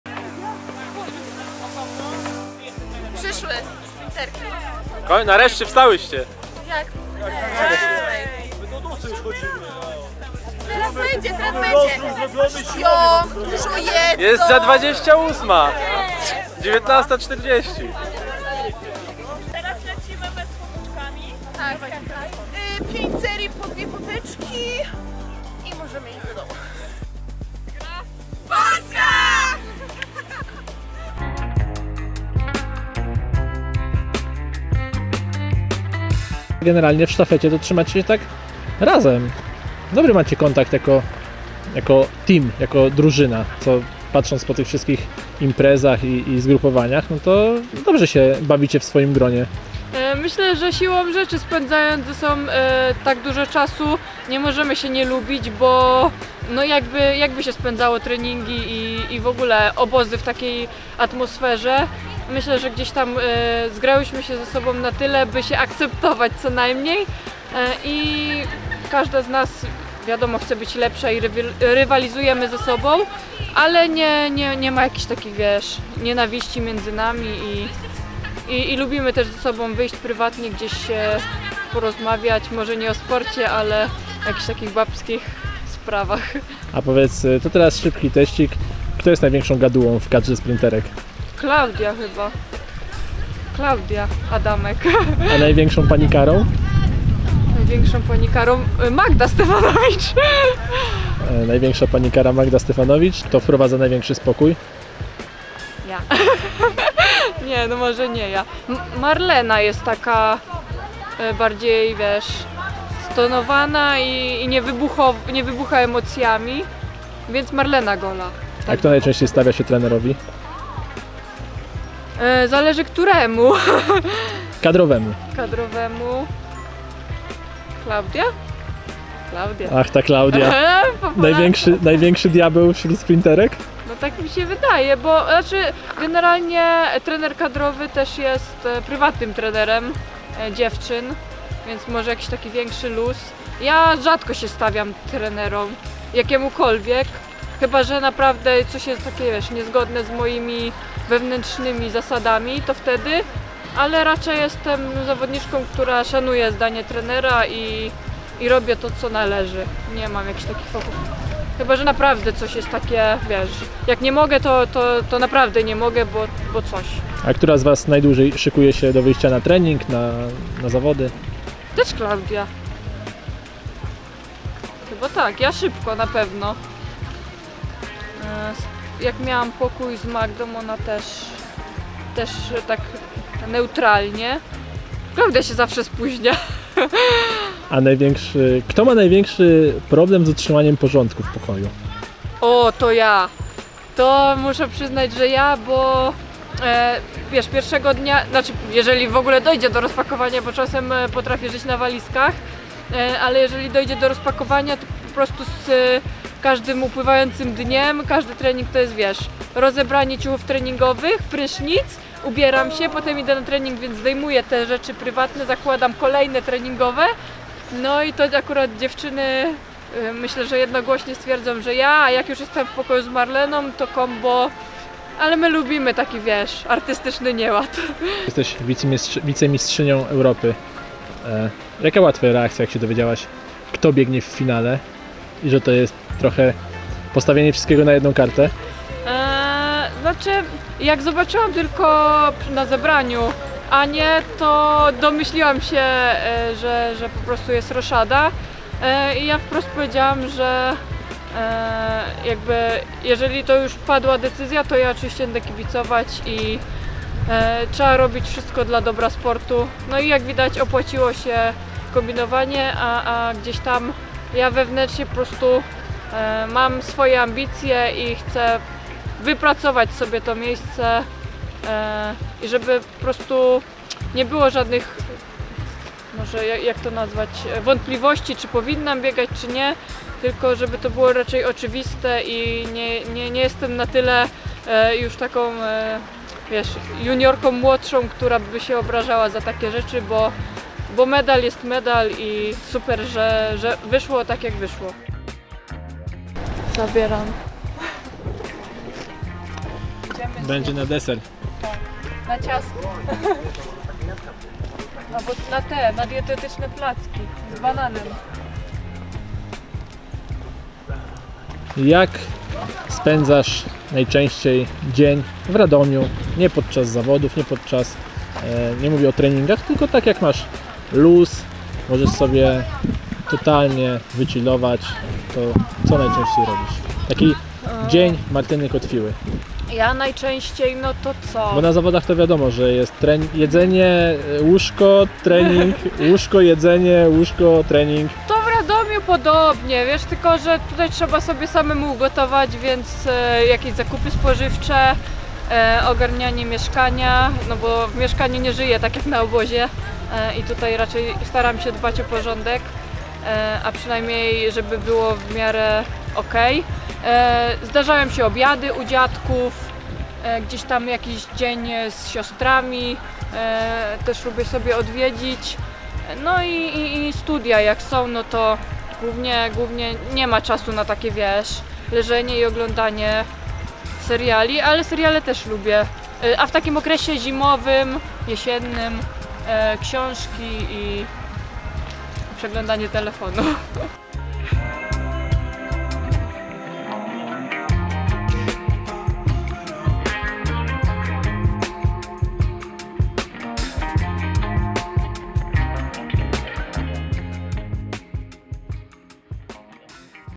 Królowe BAŁAGANU, GADUŁA i PANIKARA - sztafeta 4x100 oczami Martyny KOTWIŁY!
W grupie siła chciałoby się powiedzieć patrząc na kobiecą sztafetę 4x100 metrów. Podczas wizyty w Radomiu u Marftyny Kotwiły dowiedzieliśmy się trochę jakie dziewczyny są poza bieżnią. Sprawdźcie i Wy! A my na pewno będziemy to weryfikować u pozostałych członkiń sztafety.